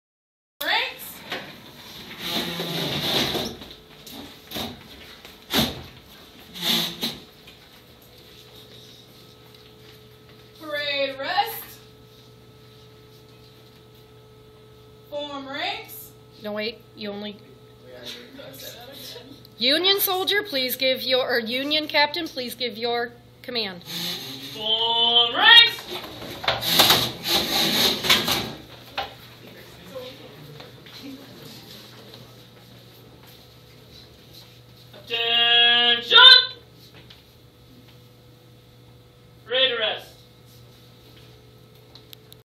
Period 6: Gettysburg
8th graders in House 8-1 reenact the Battle at Gettysburg.  May 13, 2013.  This is a short clip of the Union and Confederate Captains trying to get their troops in order.